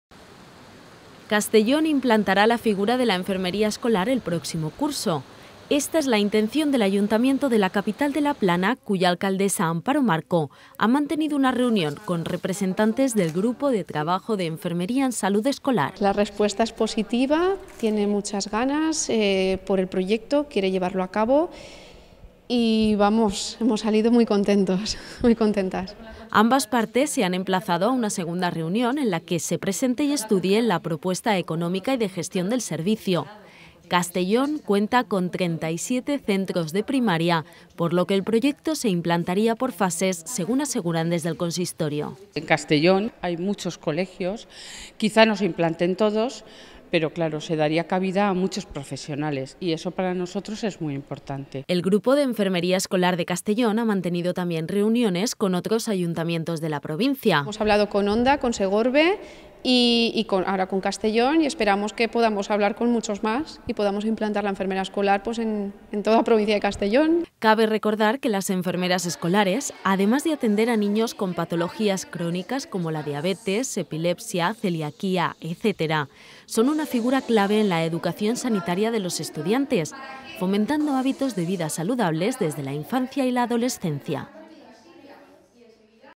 Reunión de enfermeras escolares con Ayto. Castellón | Cecova TV
Castellón tiene previsto implantar la figura de la enfermera escolar en sus centros de Primaria a partir del próximo curso. Así lo manifestó la alcaldesa, Amparo Marco, a las representantes del Grupo de Trabajo en Salud Escolar de CECOVA (ENSE) en una reunión conjunta.